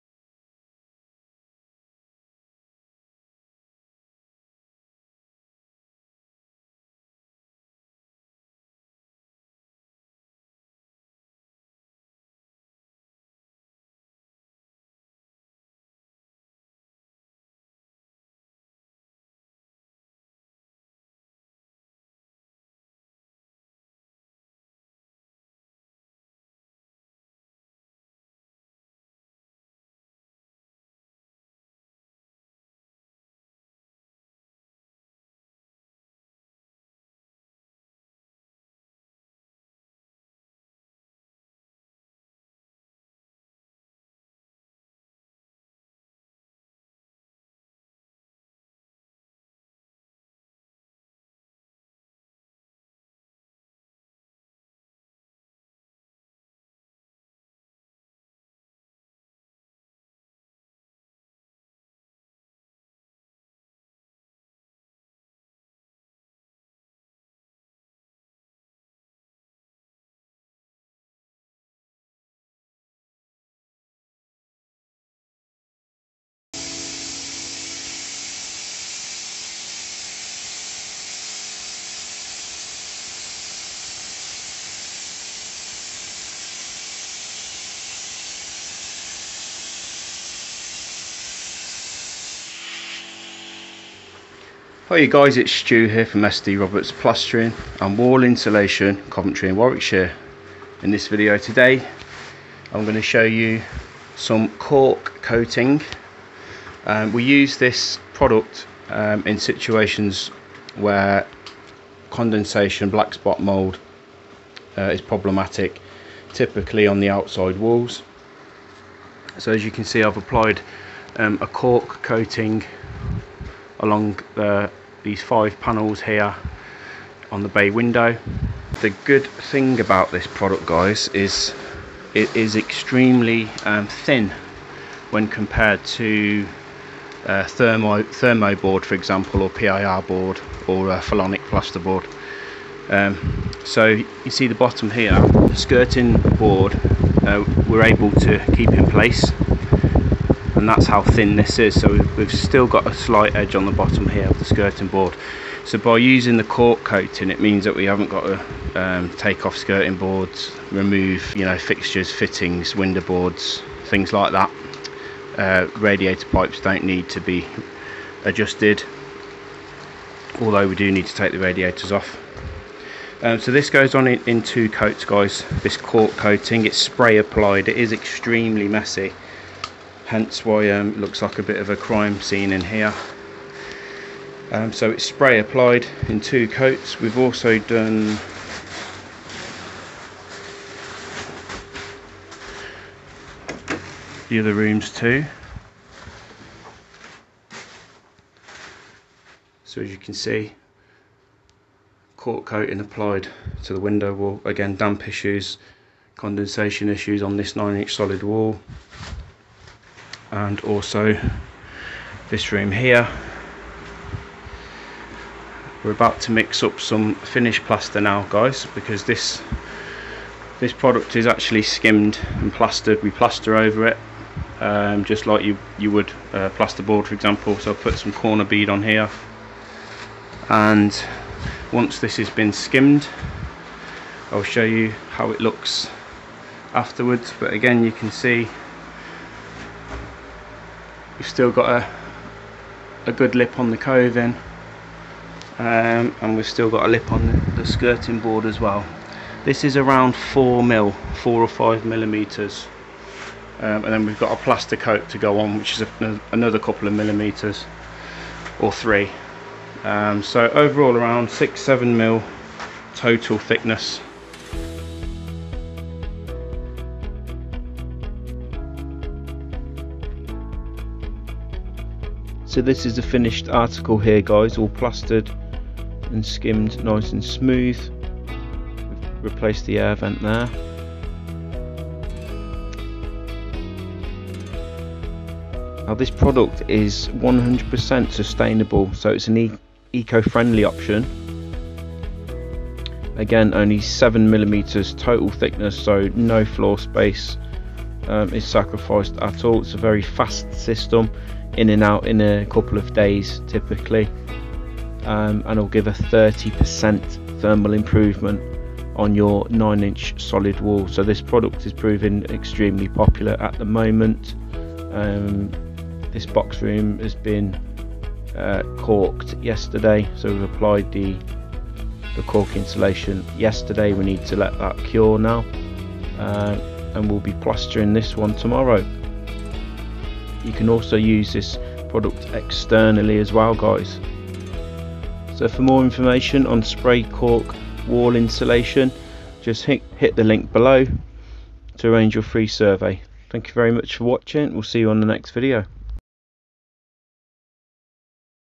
What is Spray Cork Insulation?
At only 6mm in thickness this amazing natural product completely eliminates black spot mould & condensation issues by raising the thermal resistance of the wall. No floor space is compromised & a whopping 30% heat retention is achieved

Spray Cork insulation can also be applied as a render coat for the outside & will completely transform your property while also significantly reducing heatloss on solid walls.

Other benefits of spray cork include

👉Fire resistant
👉Sound insulating
👉100% natural & sustainable
👉 Significantly reduces heat loss at only 6mm thickness